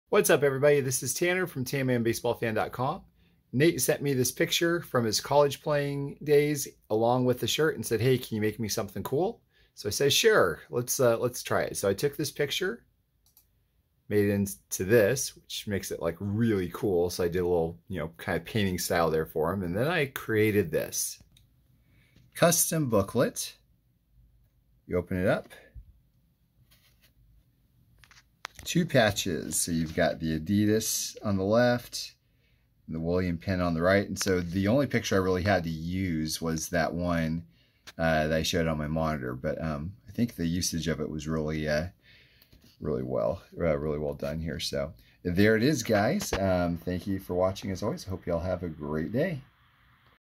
Custom Dual Patch Booklet Using a small pic and game-worn shirt!
Nate sent me a small picture of himself in his college playing days, along with a shirt.  Here is the custom booklet I created for him!